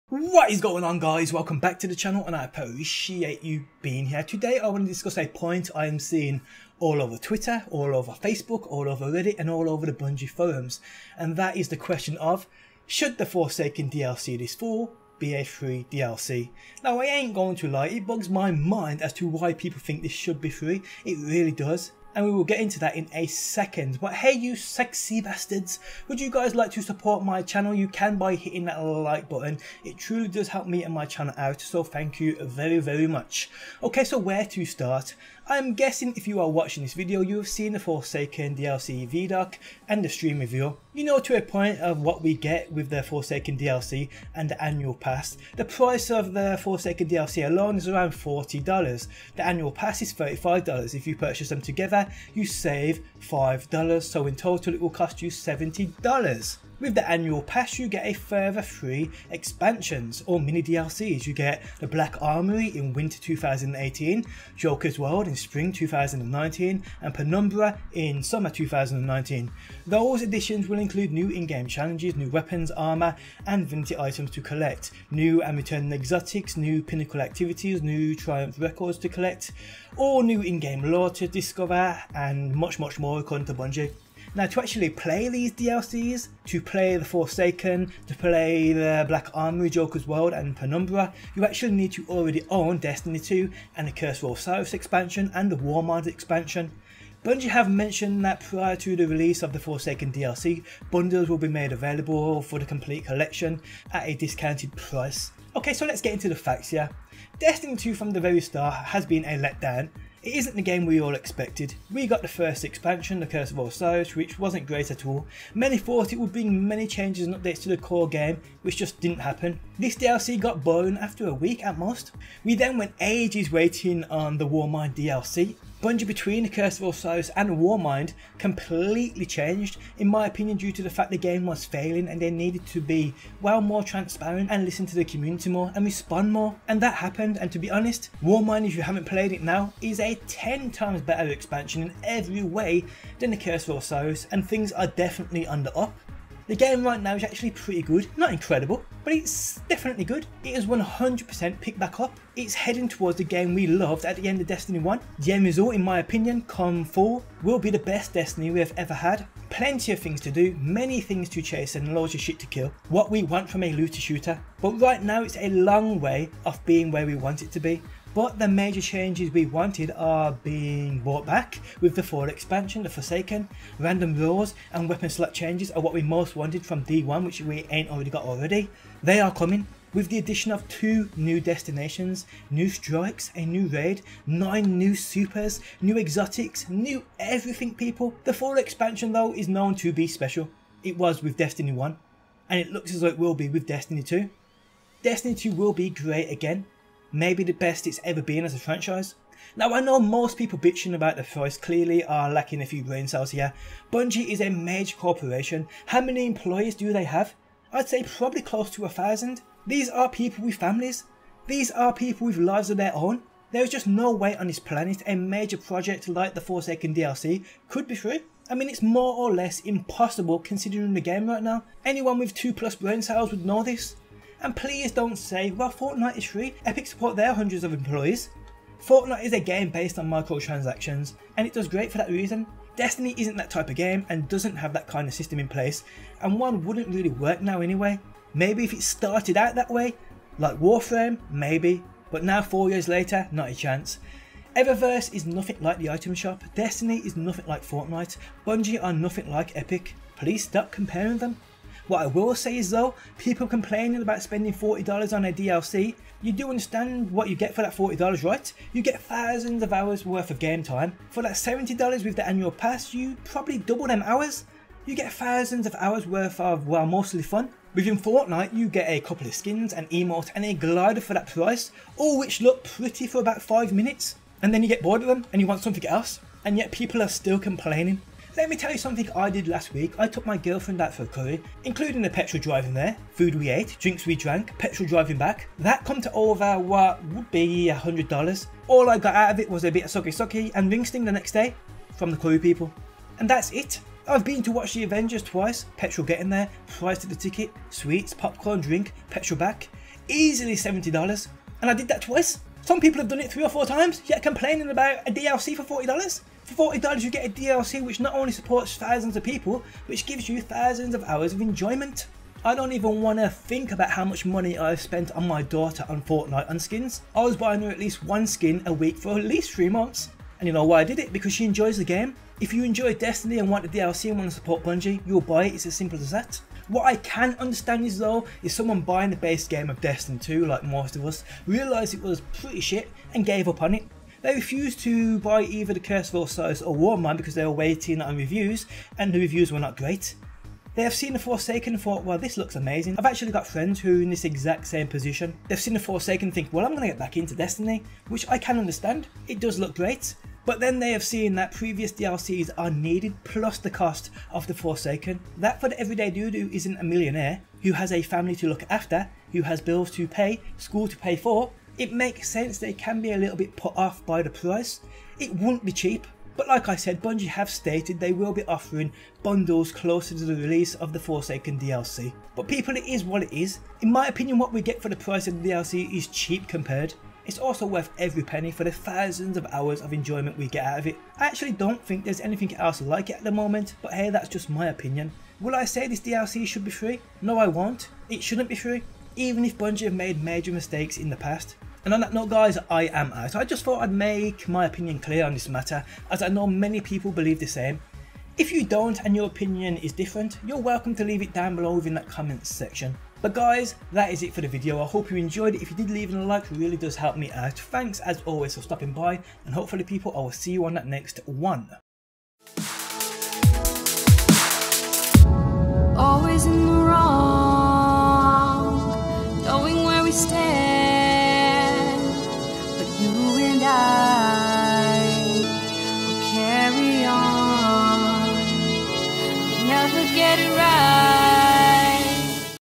People Need To Stop Bitching About The Price Of The Forsaken DLC.
My thoughts on the Destiny 2 Forsaken DLC being free.